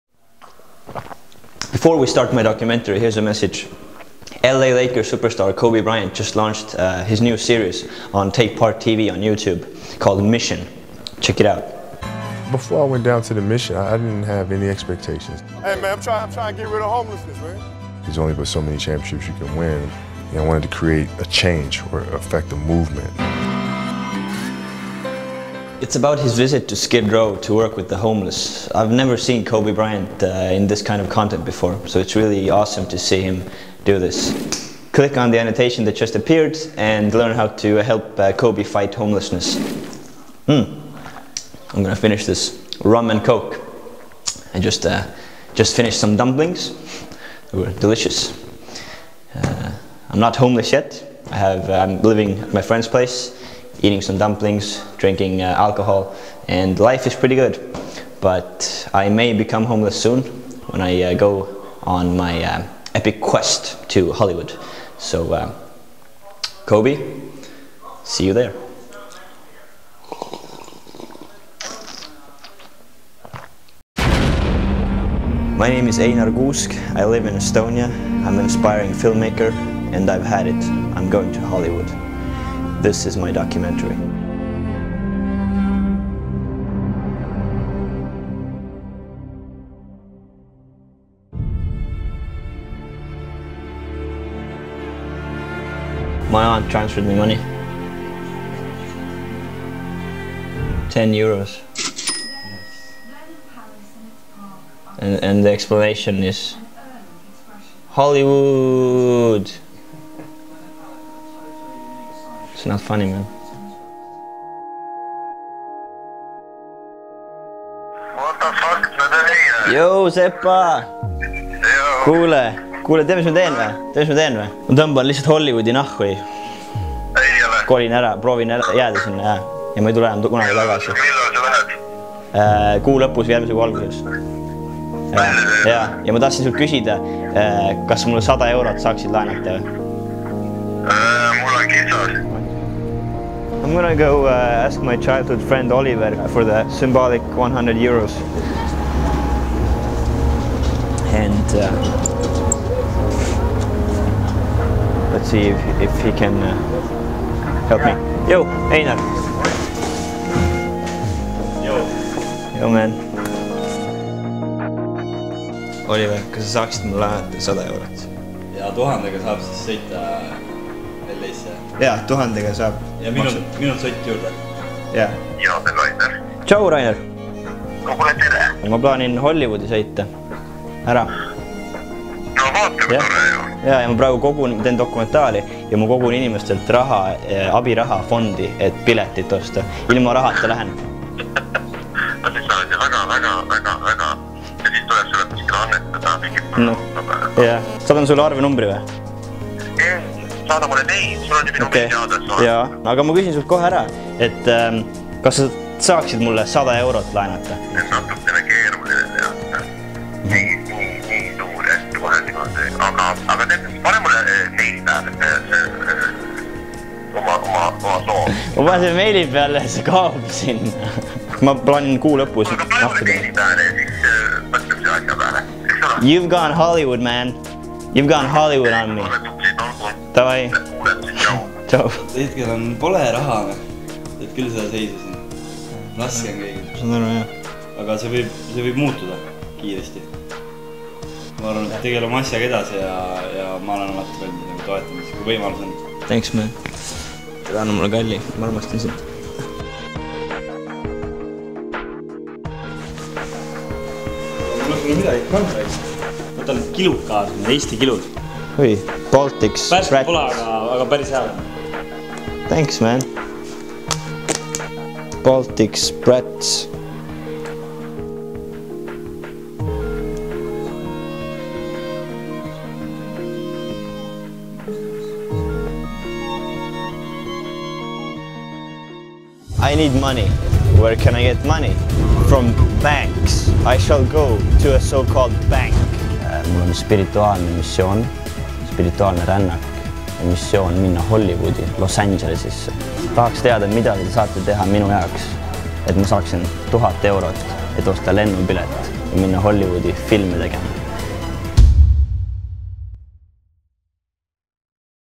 I'm Going To Hollywood - Baltic Sprats
The story continues... I made a visit to my good friends in Tallinn to ask them for money and called a few more friends.

TOTAL MONEY COLLECTED: 110 EUR